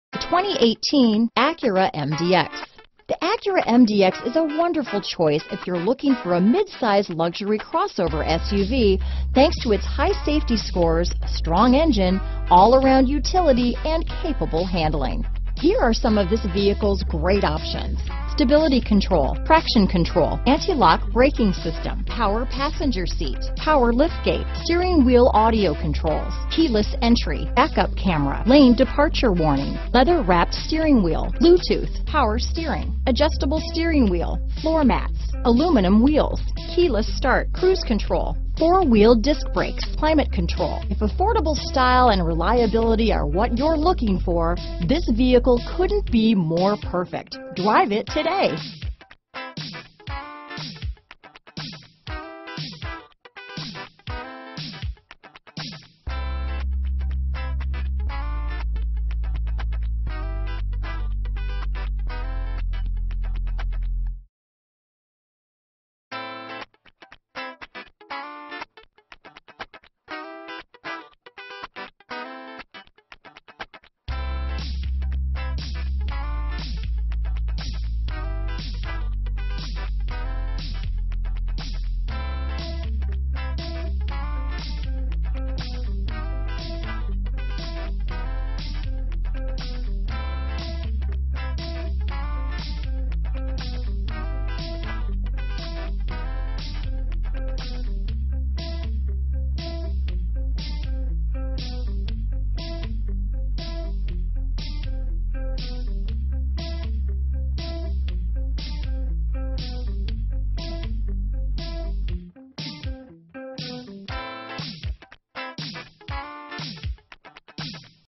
2018 Acura MDX Los Angeles, Glendale, Pasadena, Cerritos, Alhambra, CA 25278
Gray New 2018 Acura MDX available in Los Angeles, California at Sierra Acura. Servicing the Glendale, Pasadena, Cerritos, Alhambra, CA area.

Sierra Acura
1700 W Main St

Gray 2018 Acura MDX 3.5L FWD 9-Speed Automatic 3.5L V6 SOHC i-VTEC 24VRecent Arrival! 27/19 Highway/City MPG**